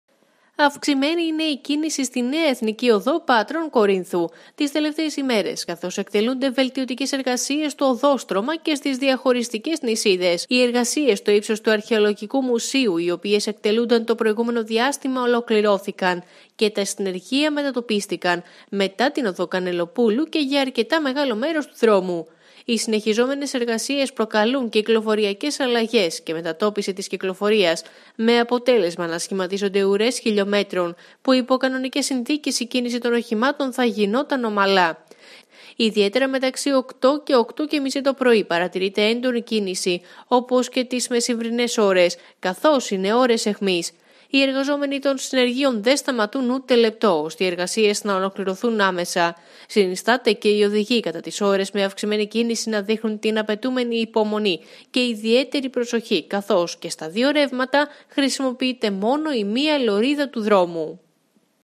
ΑΥΞΗΜΕΝΗ ΚΙΝΗΣΗ ΛΟΓΩ ΕΡΓΑΣΙΩΝ ΣΤΗΝ ΕΘΝΙΚΗ ΟΔΟ ΠΑΤΡΩΝ-ΚΟΡΙΝΘΟΥ
SUPER B Ειδήσεις 07.08.20